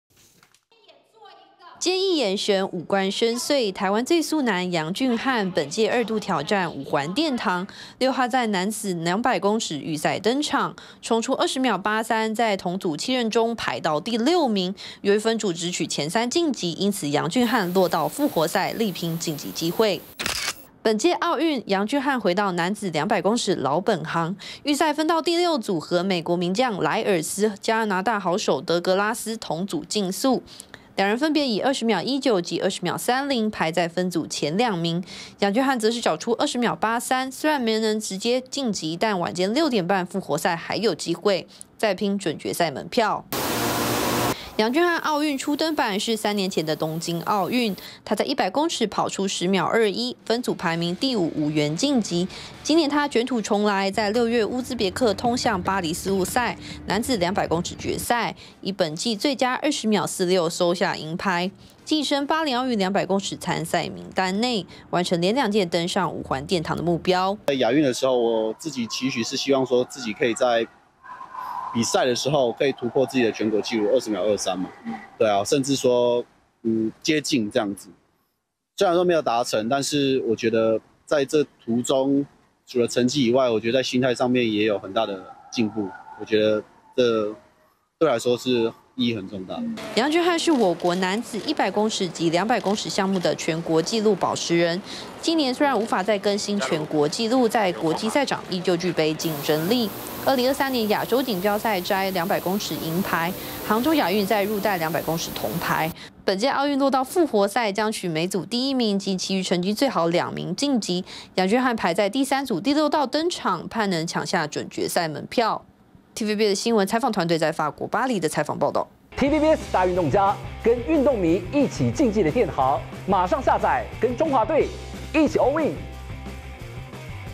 預賽「20秒83」分組第6 楊俊瀚200公尺落入復活賽｜TVBS新聞
台灣田徑隊好手楊俊瀚6號在巴黎奧運男子200公尺預賽登場，衝出20秒83，分組排名落到第6名，晚上6點半復活賽要搶準決賽門票，楊俊瀚被排到第三組第六道出賽，力拚晉級機會。